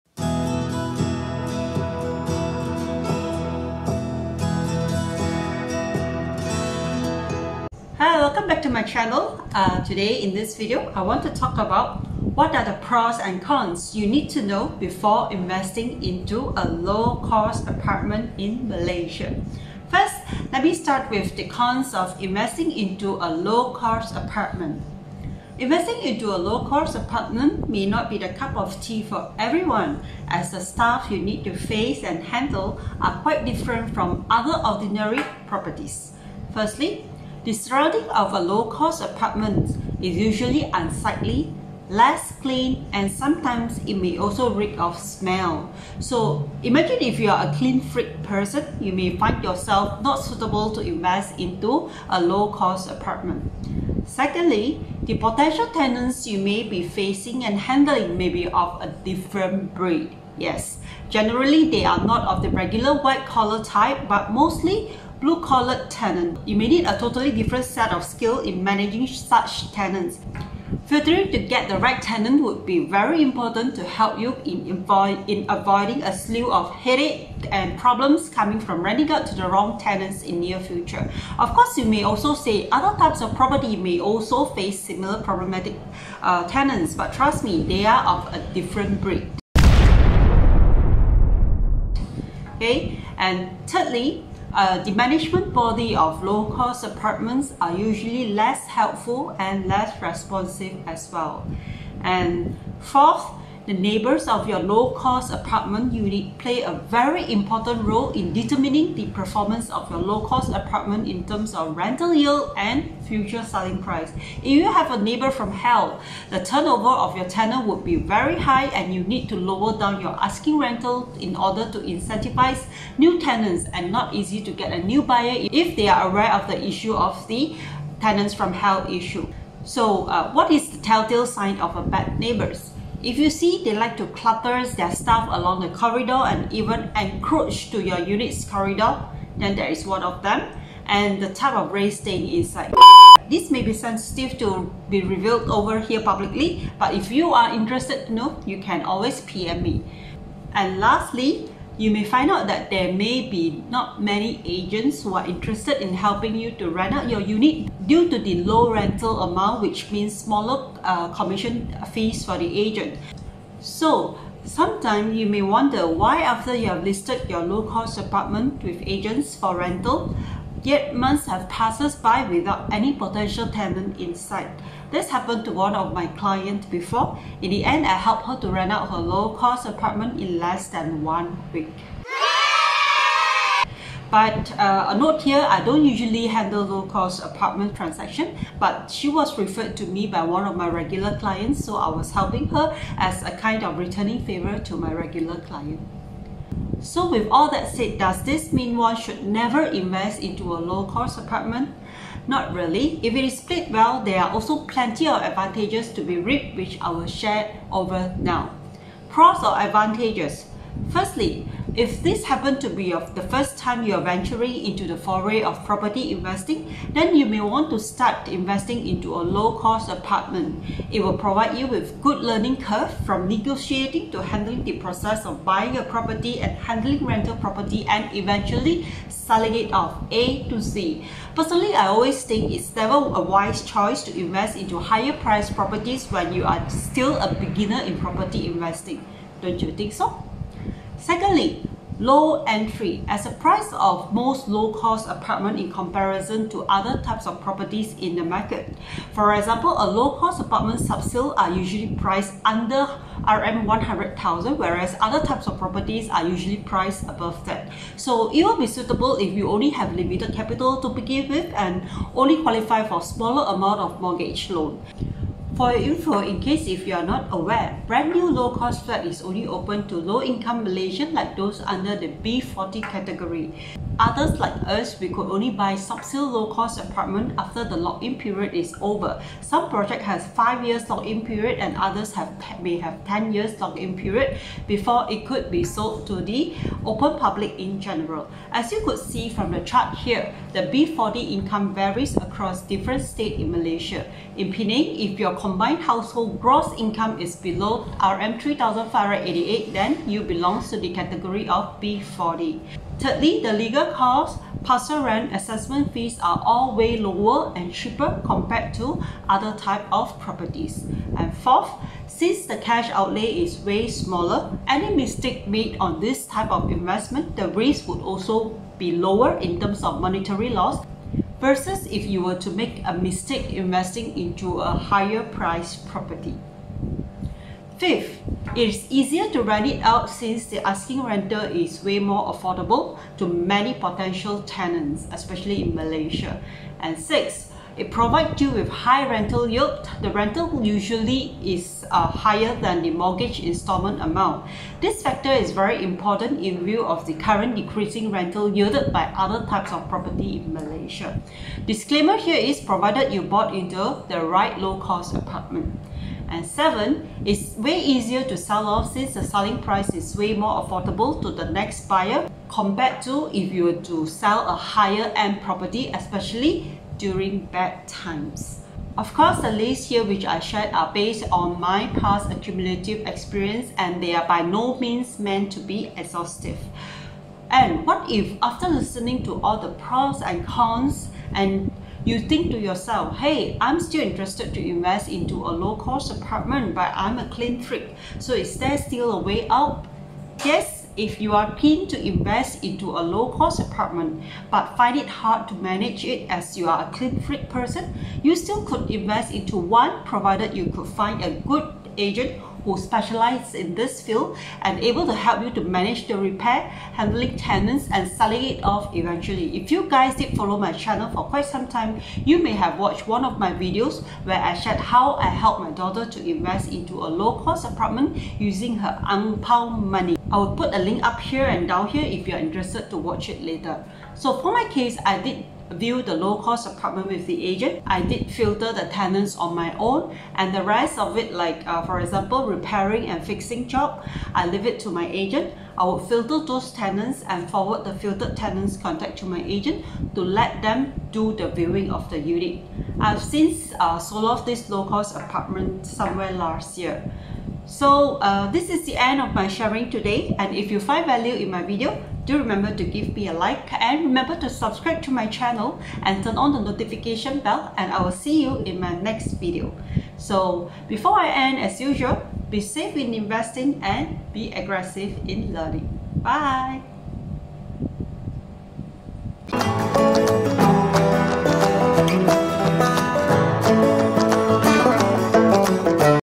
What You Should Know Before Investing into a LOW-COST Apartment in Malaysia (在投资低成本公寓之前您需要了解的利弊)
What are the pros and cons you need to know before investing into a low-cost apartment in Malaysia?
If you are interested to know, do watch this video till the end, where i will share with you based on my past accumulative experience as a property investor and property agent in real estate industry for more than a decade.